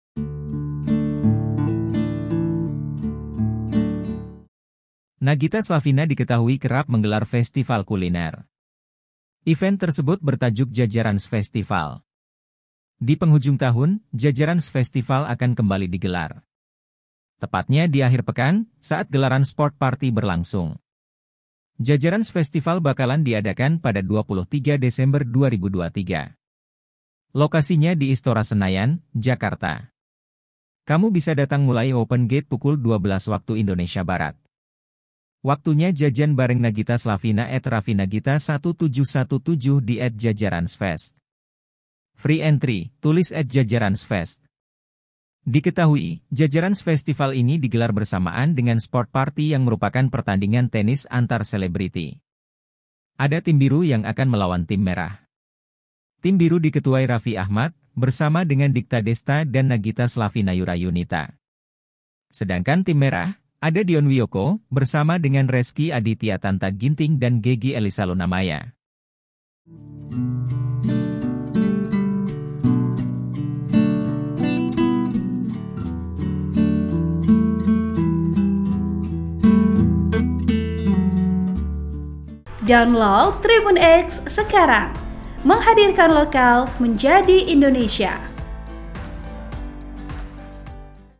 Jajarans Festival Kuliner Hadir Lagi, Masuknya Gratis Lho
TRIBUN-VIDEO.COM - Nagita Slavina diketahui kerap menggelar festival kuliner.

Event tersebut bertajuk Jajarans Festival.

Di penghujung tahun, Jajarans Festival akan kembali digelar.

Tepatnya di akhir pekan, saat gelaran Sport Party berlangsung.

Jajarans Festival bakalan diadakan pada 23 Desember 2023.

Lokasinya di Istora Senayan, Jakarta.

Kamu bisa datang mulai open gate pukul 12.00 WIB.

"Waktunya jajan bareng Nagita Slavina @raffinagita1717 di @jajaransfest. Free entry. Approved," tulis @jajaransfest.

Diketahui, Jajarans Festival ini digelar bersamaan dengan Sport Party yang merupakan pertandingan tenis antar selebriti.

Ada tim biru yang akan melawan tim merah.

Tim biru diketuai Raffi Ahmad, bersama dengan Dikta-Desta dan Nagita Slavina-Yura Yunita.

Sedangkan tim merah, ada Dion Wiyoko, bersama dengan Rezky Adhitya-Tanta Ginting dan Gege Elisa-Luna Maya.